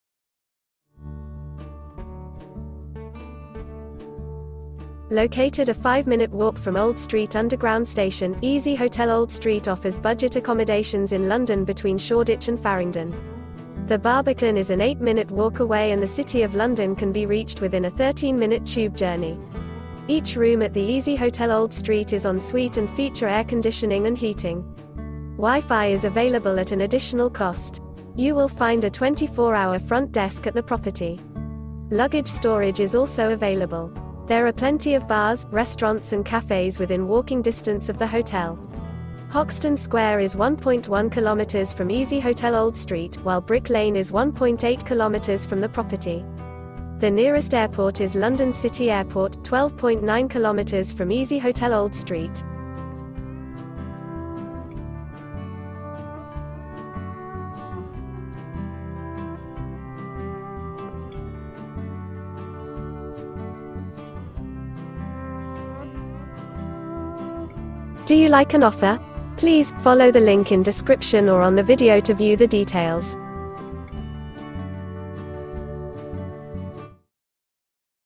easyHotel Old Street | Review Hotel in London, Great Britain
Located a 5-minute walk from Old Street underground station, EasyHotel Old Street offers budget accommodations in London between Shoreditch and Farringdon. The Barbican is an 8-minute walk away and the City of London can be reached within a 13-minute tube journey. Each room at the EasyHotel Old Street is en-suite and feature air-conditioning and heating. WiFi is available at an additional cost. You will find a 24-hour front desk at the property. Luggage storage is also available. There are plenty of bars, restaurants and cafes within walking distance of the hotel. Hoxton Square is 1.1 km from easyHotel Old Street, while Brick Lane is 1.8 km from the property. The nearest airport is London City Airport, 12.9 km from easyHotel Old Street.